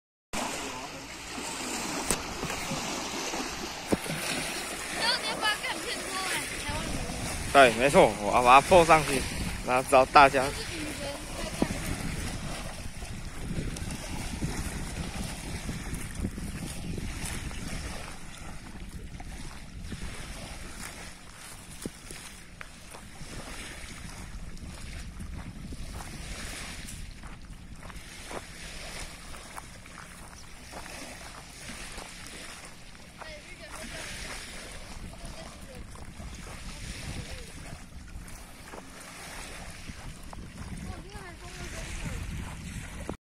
臺灣美麗的海邊
臺灣海邊習以為常的景象 但 這真的是可以習慣的東西嗎 我只是個過客 用鏡頭紀錄畫面 但各位能做的有更多 從不亂丟垃圾做起 我不是什麼正人君子 但我至少不會亂丟垃圾到海裡 欸地球真的只有這一個 這些垃圾也會藉由食物鏈回到人體 就算不為了自己 也為了族群著想好不好 算了 反正又沒人看得見 我打再多 還不是一樣有人會亂丟 執法者 你們不管管嗎 道德 不能再提升點嗎
（好吧 情緒有點激動 忘記打標點符號請見諒